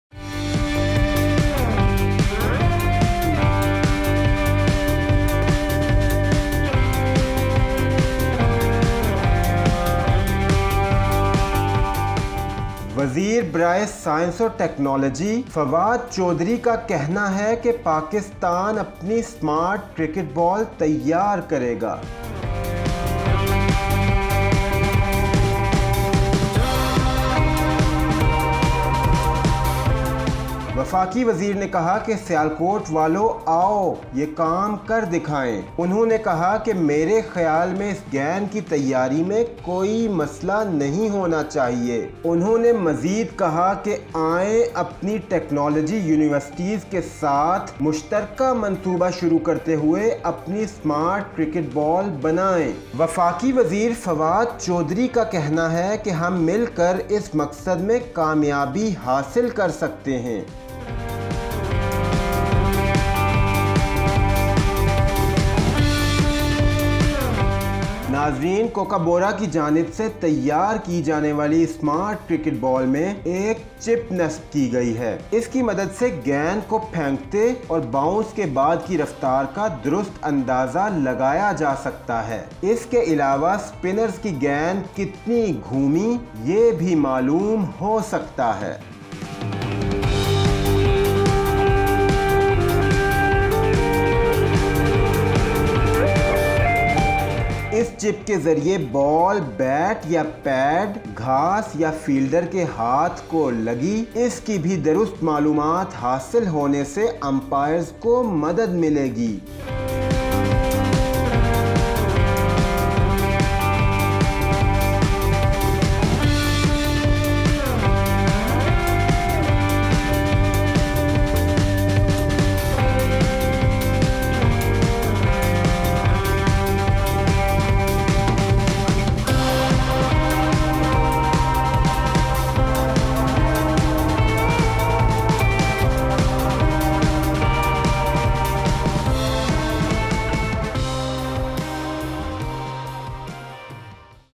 Smart Cricket Ball | Pakistan | Info Hut
Pakistan vows to develop smart cricket ball۔

Minister for Science and Technology Fawad Chaudhry has said that Pakistan will develop its own smart cricket ball. Federal Minister says that together we can achieve this goal.

The smart cricket ball, developed by Kookaburra, has a chip that can be used to accurately estimate the speed at which the ball is thrown and after the bounce, as well as how far the spinner's ball has rotated.
The chip will tell whether the ball touched bat or pad, grass or fielder's hand, it will also help the umpires to get accurate information.